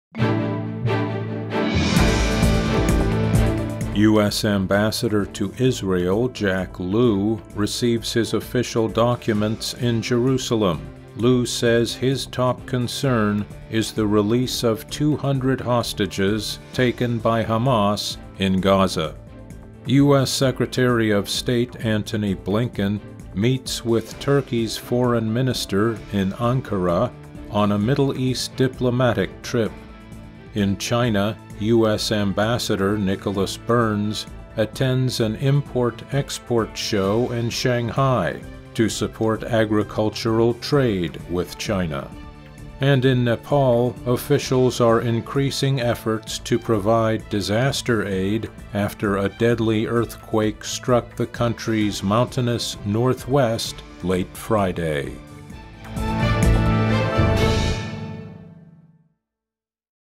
VOA60: November 6, 2023
U.S. ambassador to Israel, Jack Lew, receives his official documents in Jerusalem. Lew says his top concern is the release of 200 hostages taken by Hamas in Gaza.

U.S. Secretary of State Antony Blinken meets with Turkey's foreign minister in Ankara, on a Middle East diplomatic trip.

In China, U.S. Ambassador Nicholas Burns attends an import/export show in Shanghai to support agricultural trade with China.

And in Nepal, officials are increasing efforts to provide disaster aid after a deadly earthquake struck the country’s mountainous northwest late Friday.